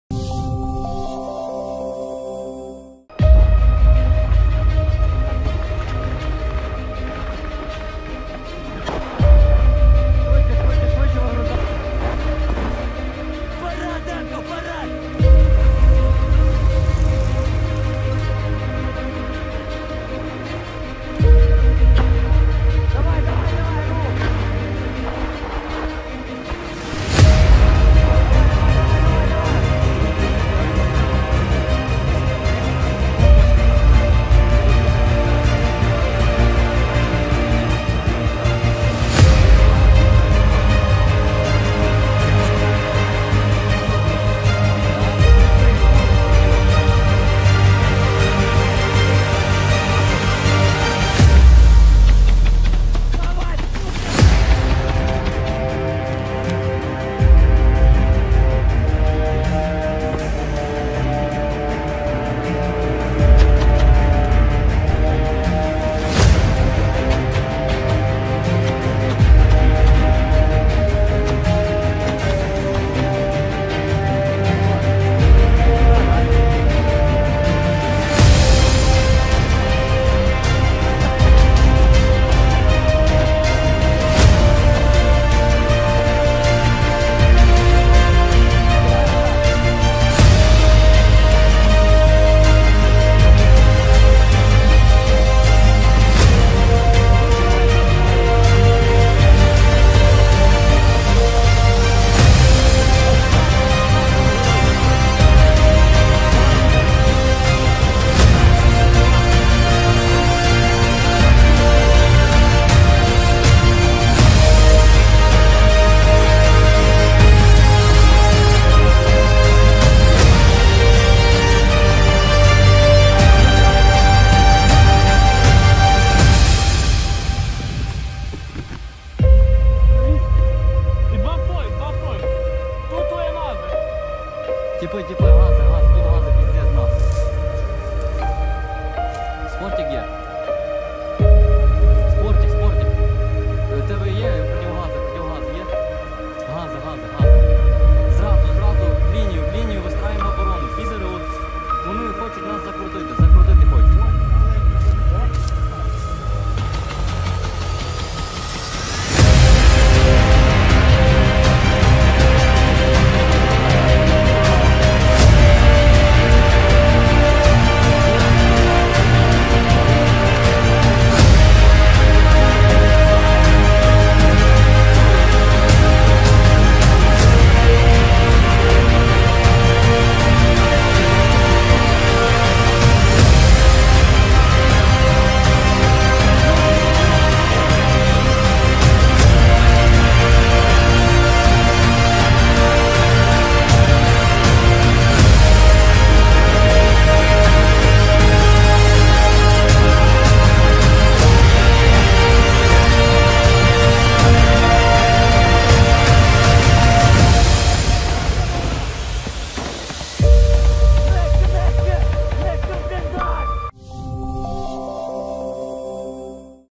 "Kill them all, don't stop" - Quick operation footage of the 3rd brigade in Kharkov forest
Fighters of the 3rd assault brigade of the Ukrainian armed forces have attacked the Russian army position located in the forest strip in Kharkiv region. Arriving in the area in an armored vehicle, the fighters advanced quickly, firing small arms. As a result of the attack that lasted until the night hours, the invaders who were hiding in the bunkers in the area were killed and the position was cleared.